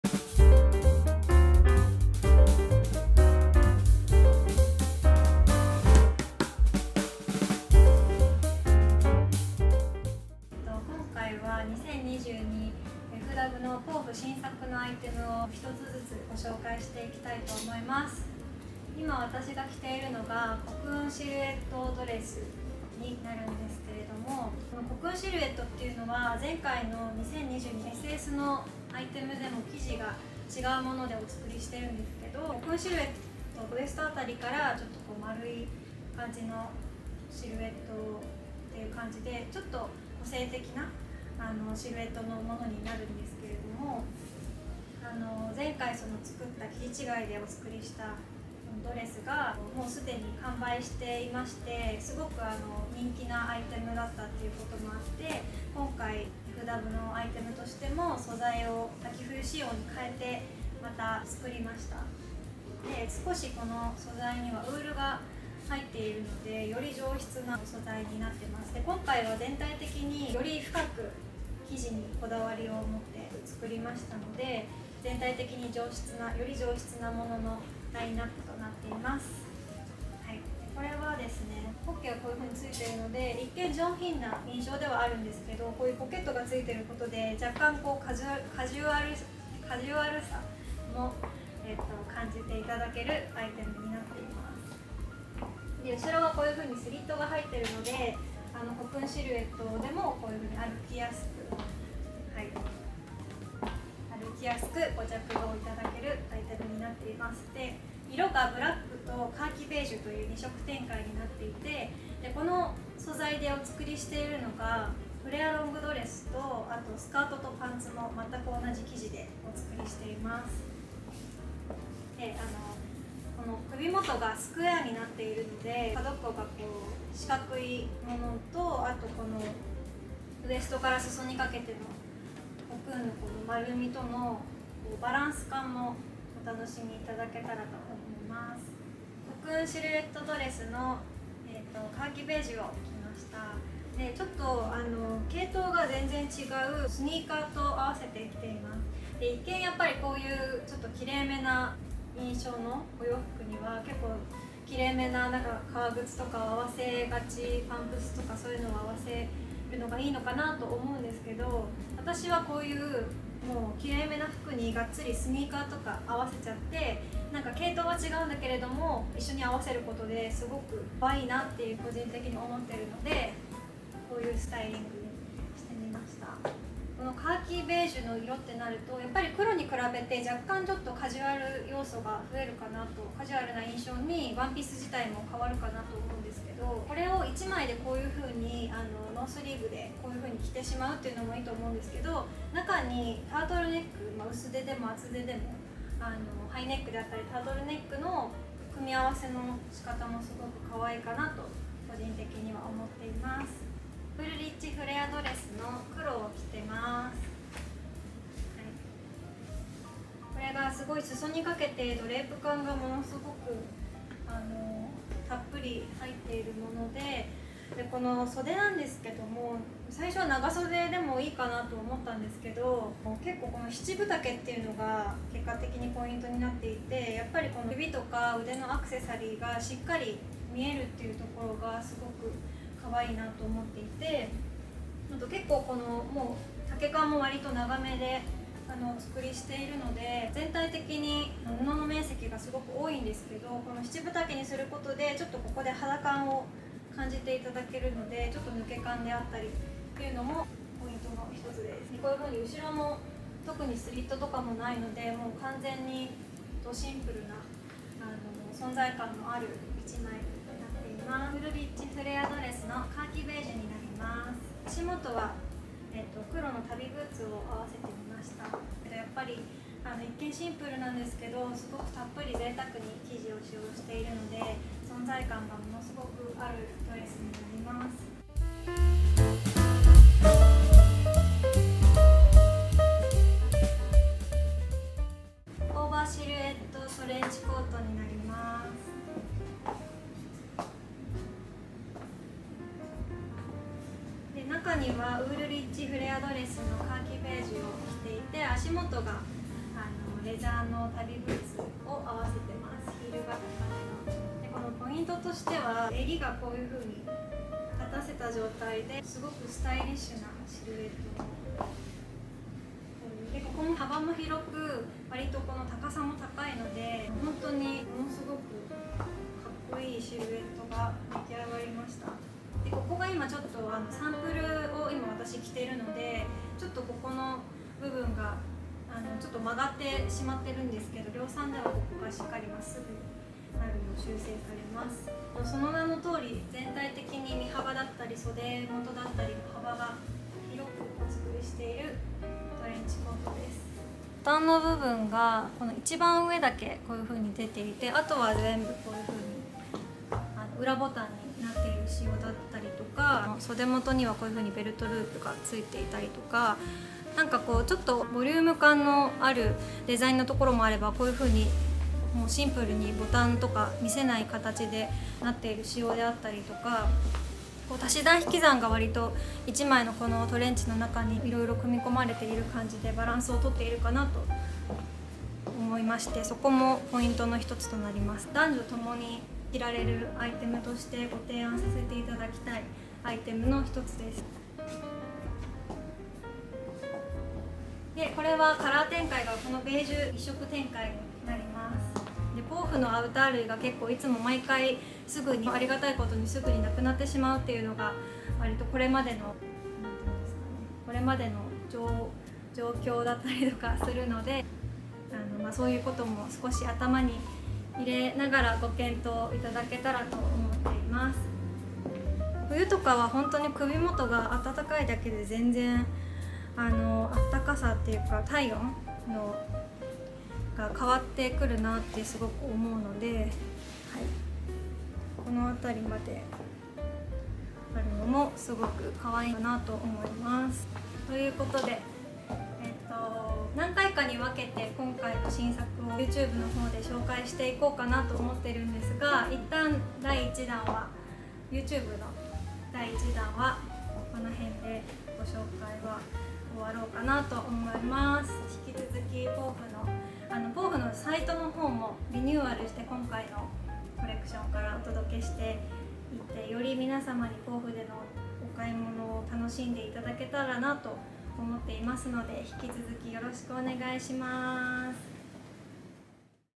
PoFF 2022 FALL WINTER LOOK #1
9/9(金) 21:00〜販売開始となる、PoFF 2022 FALL WINTERアイテムの中からまずは3アイテムご紹介致します🎥

▪︎Wool rich cocoon dress
Material : Wool50% Polyester50%
Color : Black / Khaki beige
Size : 0/1 (H160cm/Size1)
Price : ¥23,000 (taxin)

▫︎Wool rich flare dress
Material : Wool50% Polyester50%
Size : 0/1 (H160cm/Size1)
Price : ¥29,700 (taxin)

▪︎Over silhouette trench coat
Material : Polyester100%
Color : Beige
Size : F
Price : ¥34,980 (taxin)